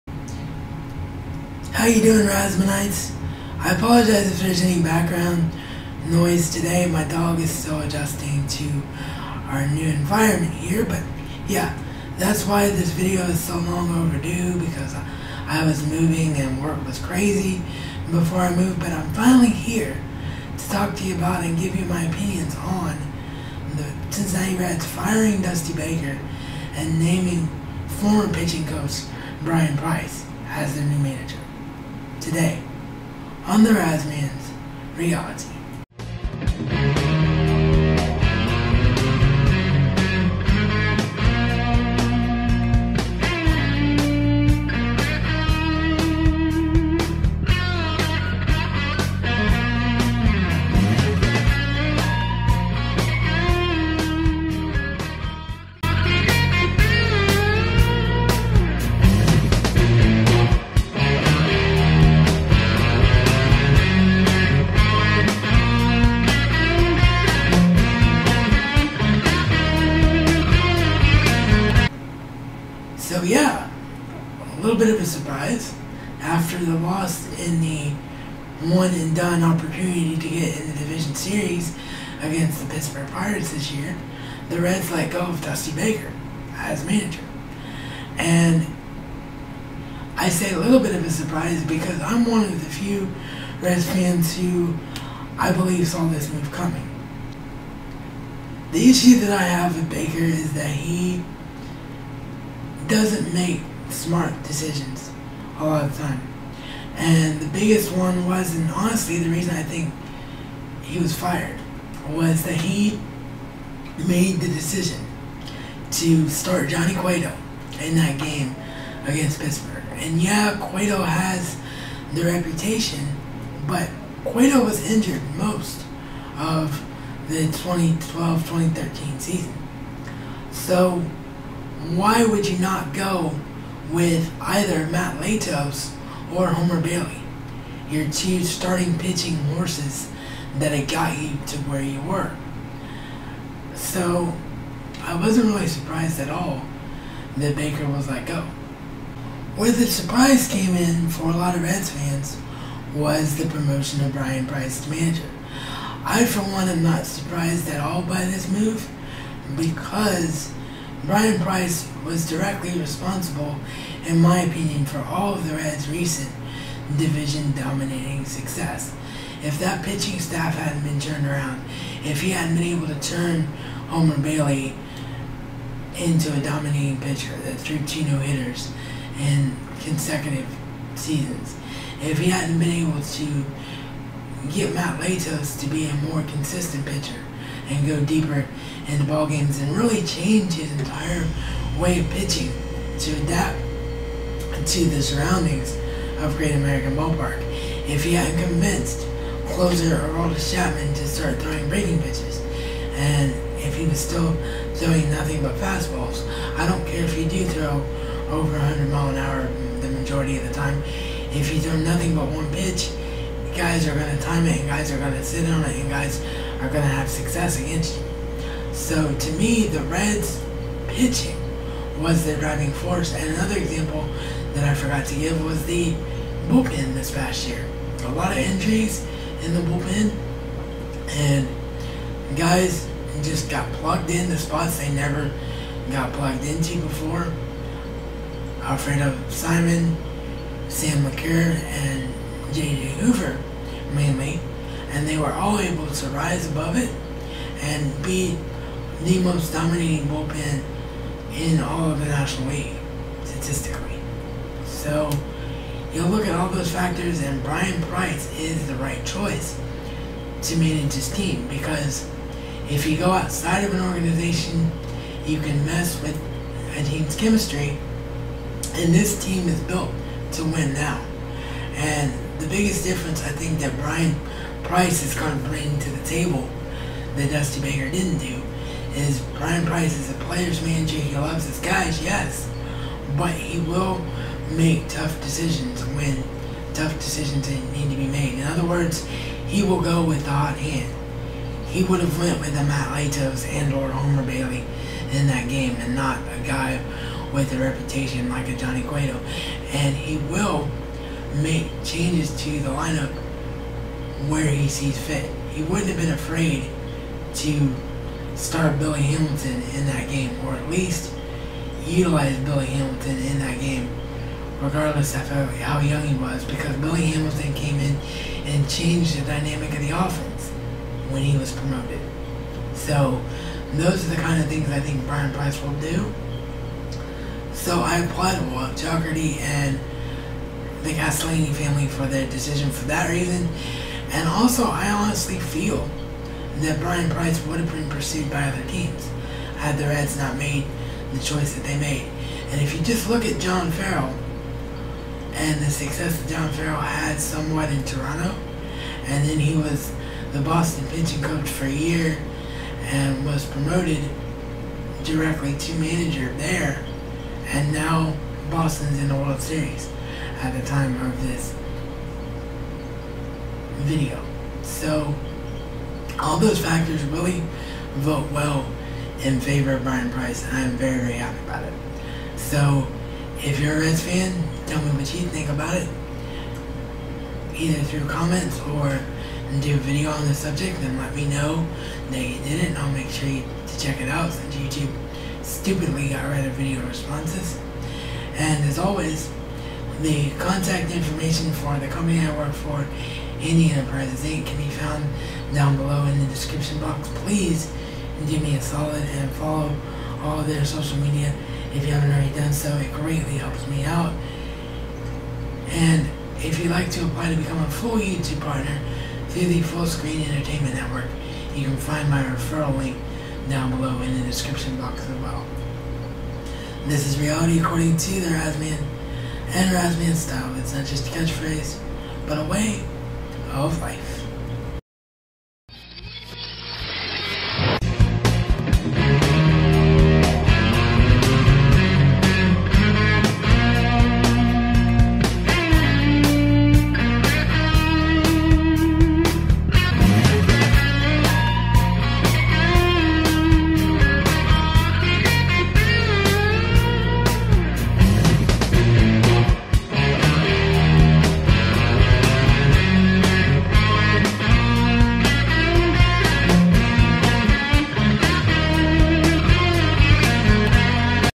Baker Out Price In My Thoughts
I created this video & own all rights to all content in this video.
October 31, 2013 Baker Out Price In My Thoughts. My thoughts on the Cincinnati Reds firing Dusty Baker as manger & naming Bryan Price as his successor.